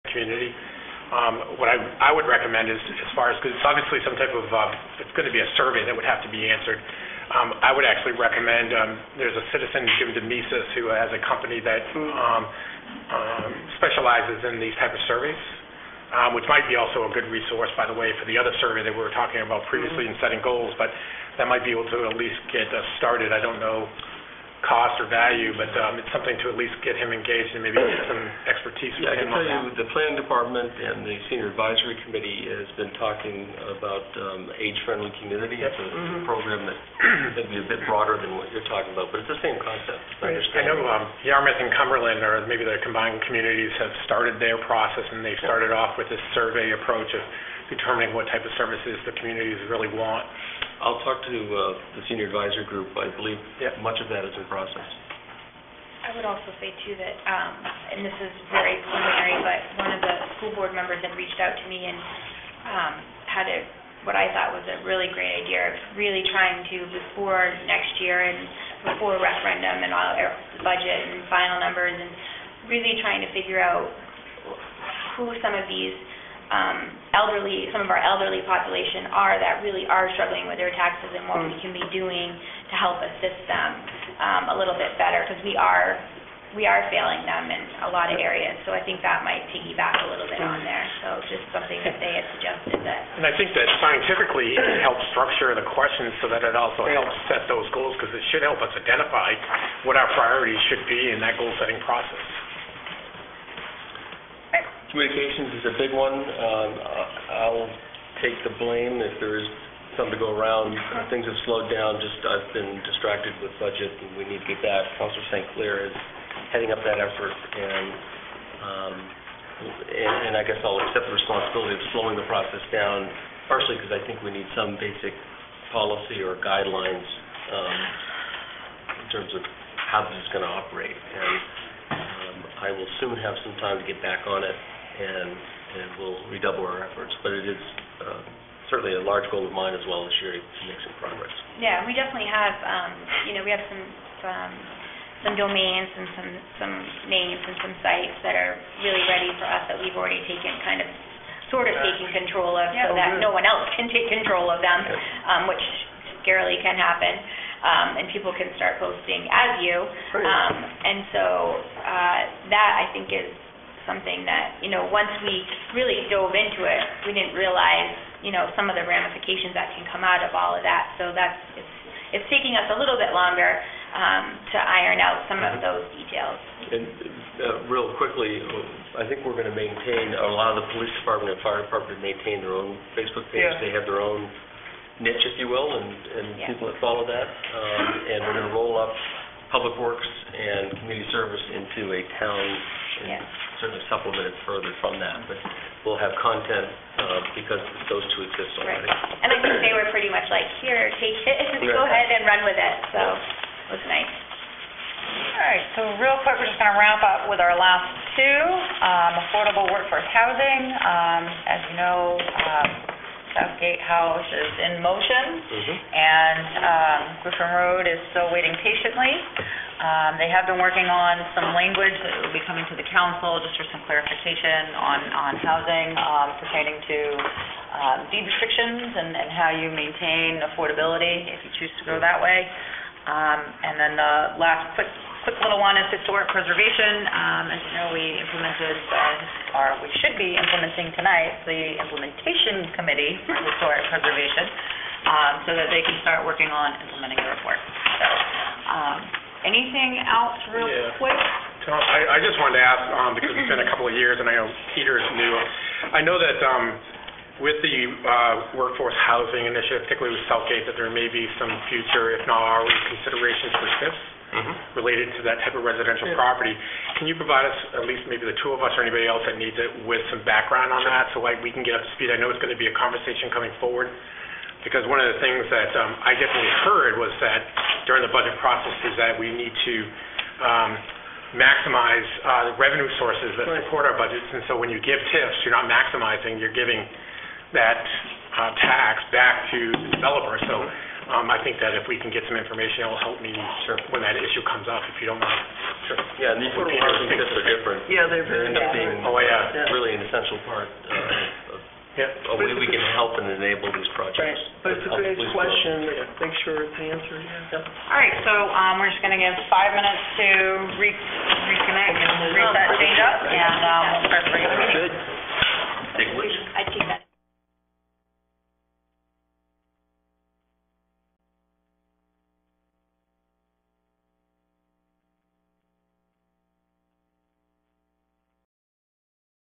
Town Council Workshop, June 17, 2015, Scarborough, Maine (partial)
Town Council Workshop, June 17, 2015, Scarborough, Maine meeting. Joined in progress due to technical difficulties, 5 minutes, 29 seconds.